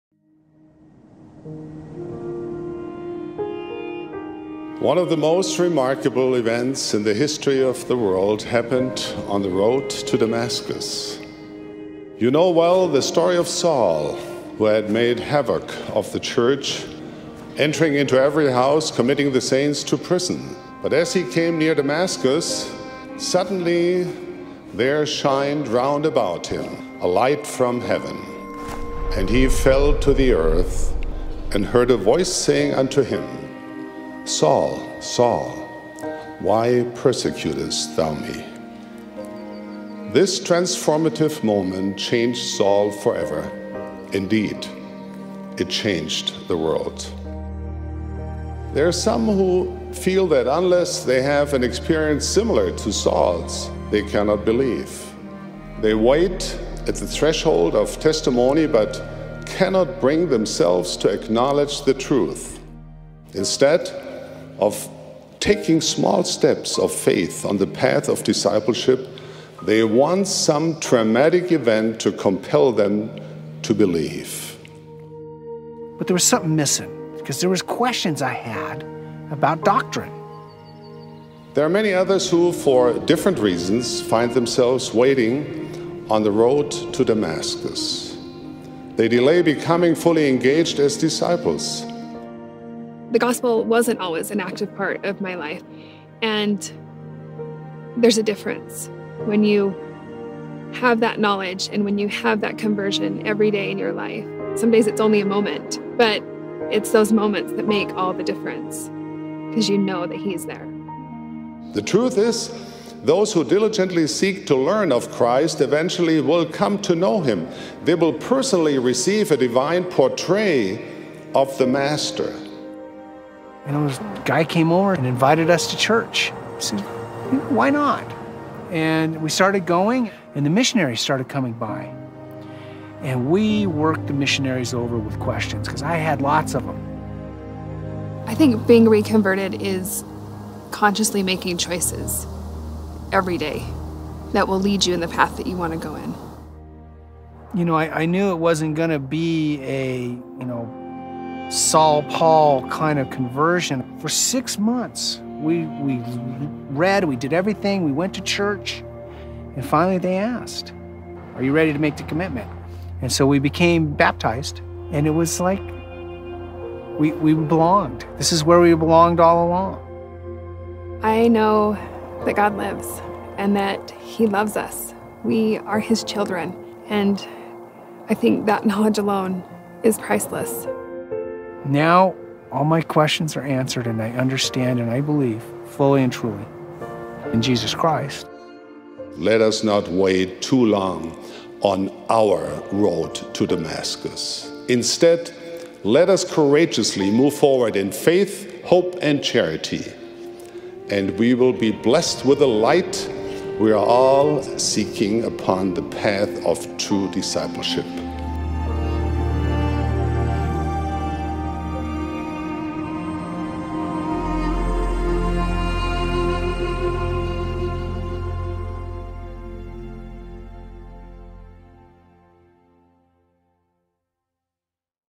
Waiting on Our Road to Damascus
Those who diligently seek to learn of Christ will eventually come to know Him. Read President Uchtdorf's full address here: